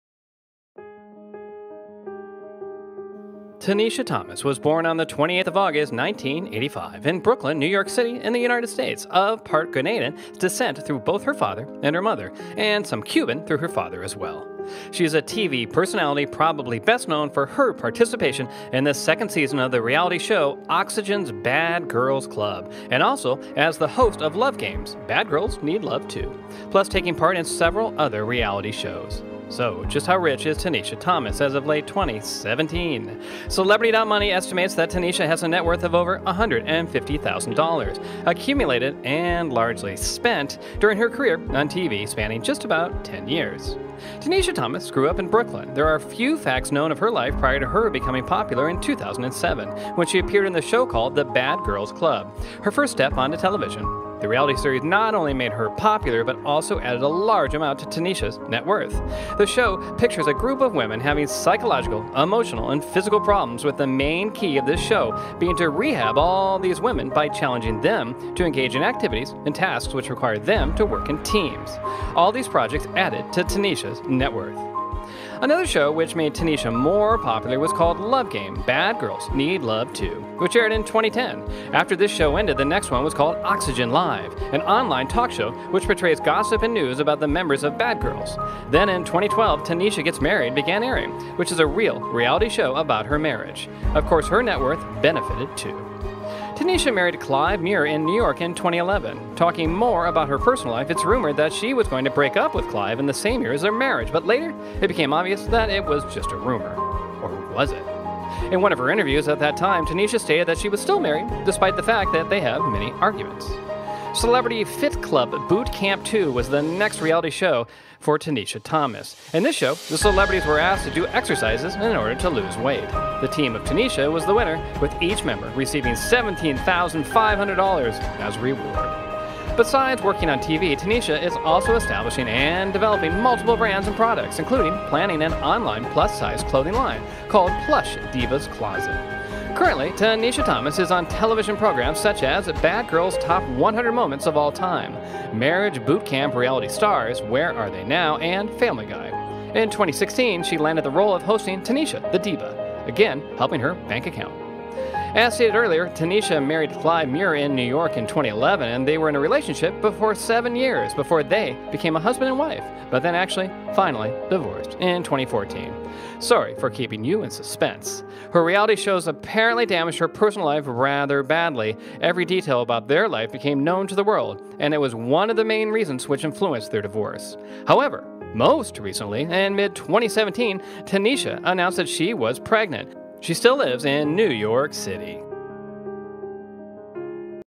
Tanisha Thomas Net Worth & Bio - Amazing Facts You Need to Know
Tanisha Thomas was born on 28 August 1985, in Brooklyn, New York City USA, of part-Grenadan decent through both her father and mother, and some Cuban through her father as well. She is a TV personality, probably best known for her participation in the 2nd season of the reality show "Oxygen's Bad Girls Club", and also as the host of “Love Games: Bad Girls Need Love Too”, plus taking part in several other reality shows.
So just how rich is Tanisha Thomas, as of late 2017? Celebrity dot money has estimated that Tanisha has a net worth of over 150,000 dollars, accumulated (and largely spent) during her career on TV spanning just about 10 years.
Tanisha Thomas grew up in Brooklyn: there are few facts known of her life prior to her becoming popular in 2007, when she appeared in the show called “The Bad Girls Club”, her first step onto television. This reality series not only made her popular, but also added a large amount to Tanisha's net worth. The show pictures a group of women having psychological, emotional and physical problems, with the main key of this show being to rehabilitate all those women by challenging them to engage in activities and tasks which require them to work in teams. All these projects added to Tanisha's net worth.
Another show which made Tanisha more popular was called " Love Game: Bad Girls Need Love Too” which aired in 2010. After this show ended, the next one was called "OxygenLIVE", an online talk show which portrays gossip and news about the members of "Bad Girls". Then in 2012, "Tanisha Gets Married" began airing, which is a REAL reality show about her marriage. Of course, her net worth benefited too.
Tanisha married Clive Muir in New York in 2011. Talking more about her personal life, it was rumored that she was going to break-up with Clive in the same year as their marriage, but later it became obvious that it was just rumor, or was it!? In one of her interviews at the time, Tanisha stated that she was still married, despite the fact that they have many arguments.
“Celebrity Fit Club: Boot Camp 2” was the next reality show in which Tanisha Thomas appeared. In this show, the celebrities were asked to do exercises in order to lose weight. The team of Tanisha was the winner, with each member receiving $17,500 as a reward.
Besides working on TV, Tanisha is also establishing and developing multiple brands and products, including planning an on-line plus-sized clothing line, called" Plush Diva's Closet".
Currently Tanisha Thomas is on such television projects as "Bad Girls Top 100 Moments of All Time", "Marriage Boot Camp Reality Stars: Where Are They Now", and "Family Guy". In 2016 she landed the role of hosting "Tanisha the Diva", again helping her bank account, if only temporarily.
As stated earlier, Tanisha married Clive Muir in New York in 2011. They were in a relationship for seven years before they became a husband and wife, but then actually, finally, divorced in 2014 - sorry for keeping you in suspense! Her reality shows apparently damaged her personal life rather badly. Every detail about their life became known to the world, and it was one of the main reasons which influenced their divorce.
However, MOST recently. in mid-2017 Tanisha announced that she was pregnant, but so far not the name of the father. She still lives in New York City.